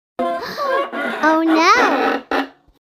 My Reaction To That @LexiHoshikoTheCuteGirlAUTTP Just Delete You Commdery Post
That was not nice to say it Because Are you being a bad thing to delete her? That's not gonna be happened to it this is why you're going to be run late And also stop deleting my Comments Post

Credits To JasonTheVideomaker2007 For This Clip Video